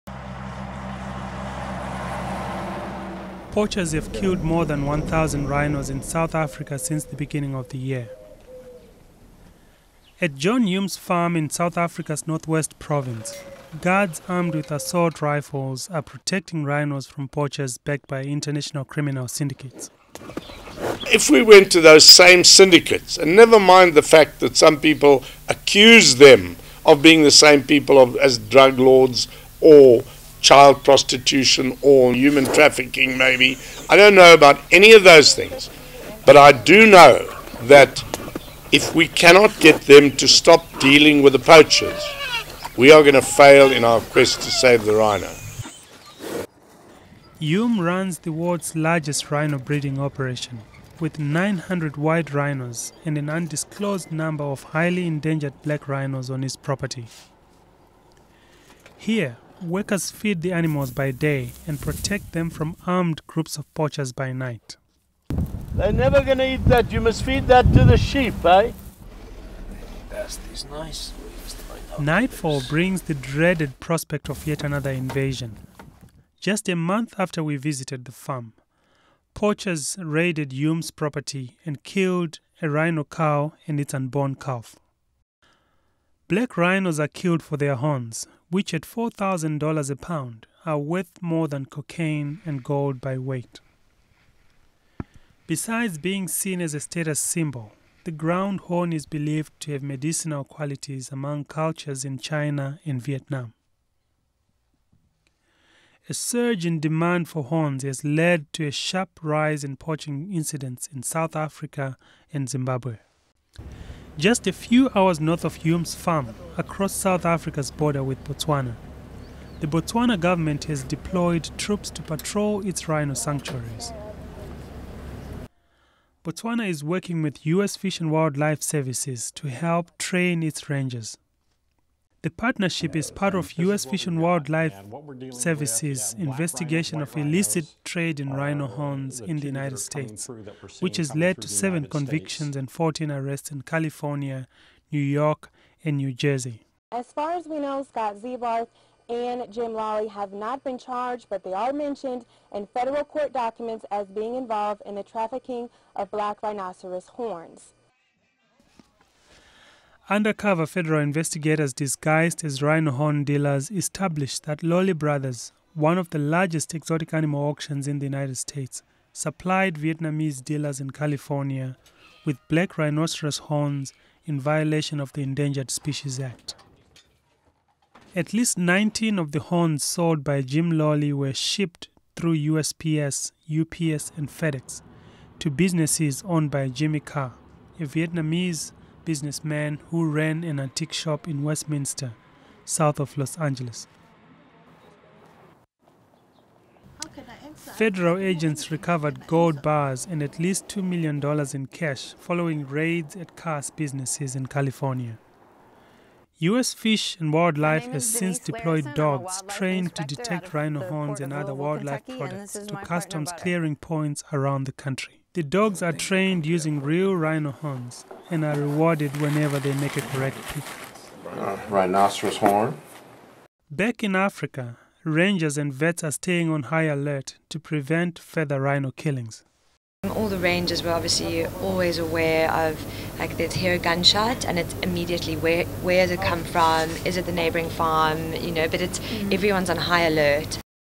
America's Exotic Animal Auctions and the Rhino Horn Black Market
This collaborative investigative reporting project takes a closer look at global rhino horn smuggling syndicates, their impact on the world's remaining natural habitats for the black rhinoceros and what is being done to stop them. - Tawanda Kanhema | Investigative Reporting Program - Berkeley Graduate School of Journalism